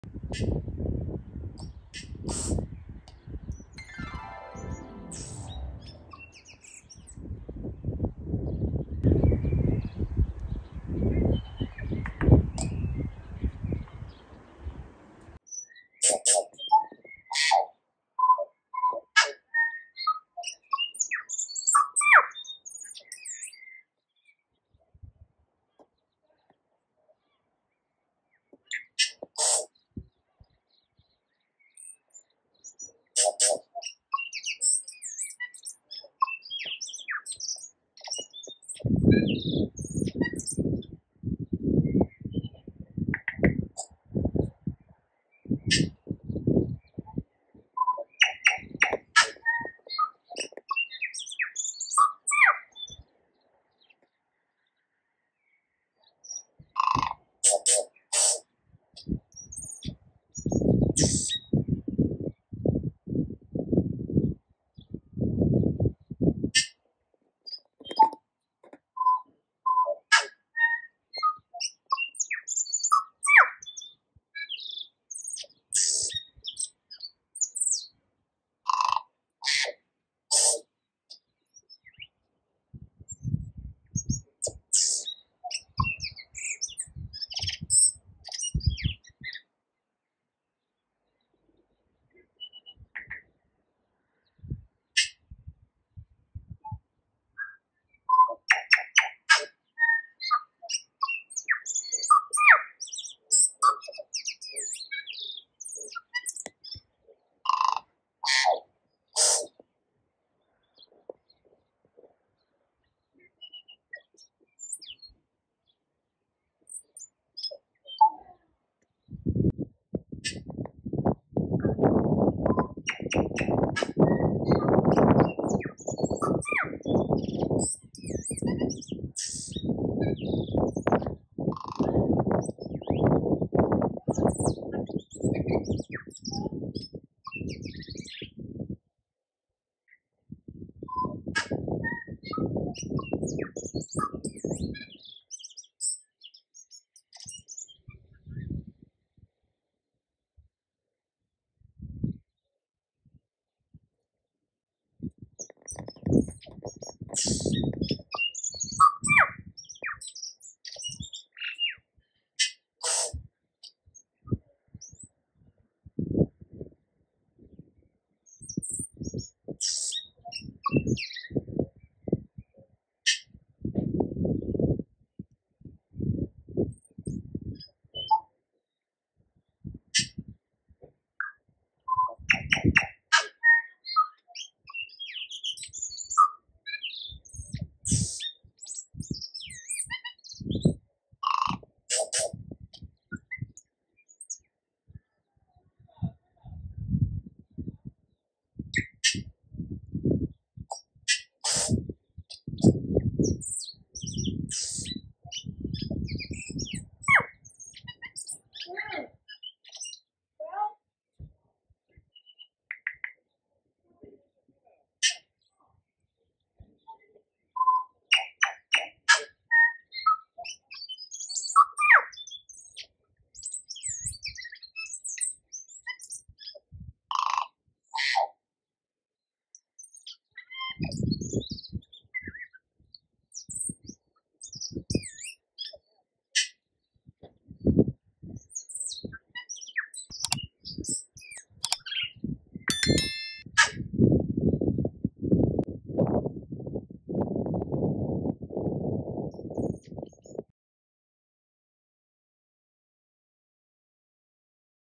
Tui birdsong
Known for its melodious and distinct calls, the Tui bird, with its iridescent feathers, creates a symphony that perfectly complements the vibrant yellow blossoms of the Kōwhai tree.